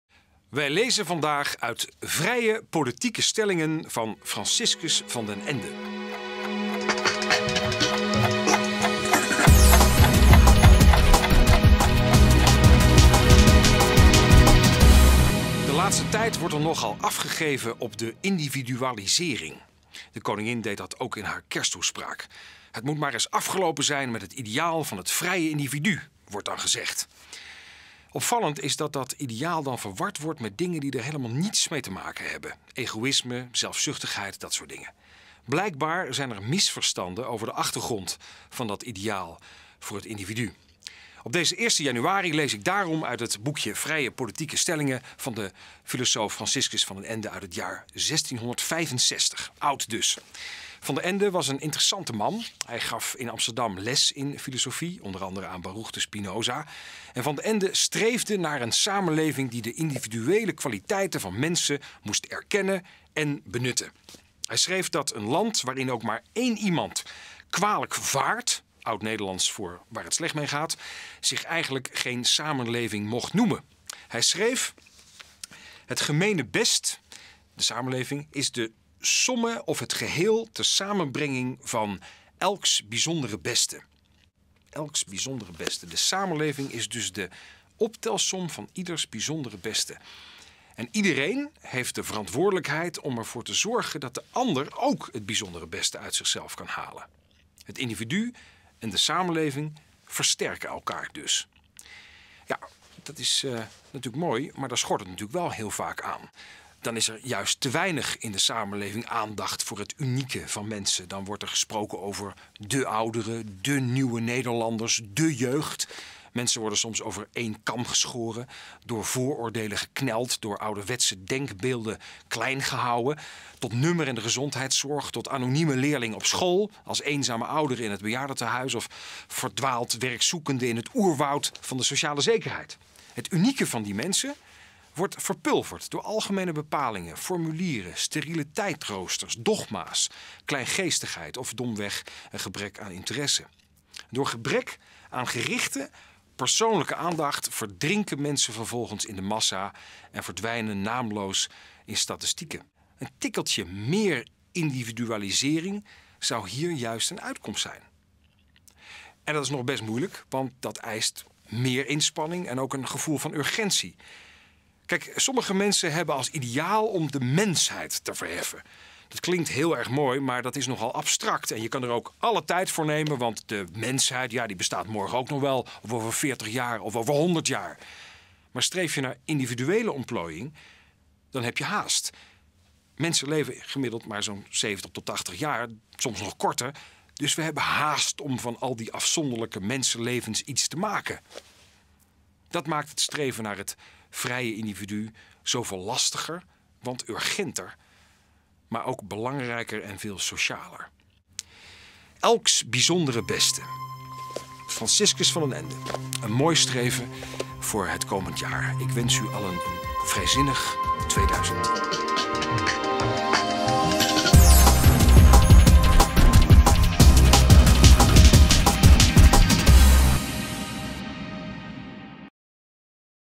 Pleidooi voor het Vrije Individu ('vrijzinnige schriftlezing')
Aan het begin van 2010 kwam deze nieuwjaars'preek'  uit. Onderwerp: vrijheid en het individu. Hij werd afgestoken door D66-kamerlid Boris van der Ham. De idealen van het vrije individu worden de laatste tijd nogal eens verkeerd uitgelegd, zoals door de Koningin bij haar kersttoespraak van 2009. Het ideaal van het vrije individu is volgens Van der Ham juist socialer en urgenter dan welk ideaal ook. Hij laat zich in deze 'vrijzinnige schriftlezing'  inspireren door een tekst uit 1665. Die was geschreven door Franciscus van den Ende, een amsterdams filosoof die o.a. lesgaf aan Spinoza.

 'Het gemeene-best is de somme of het geheele t'samen brengingh van elx bijzondere beste'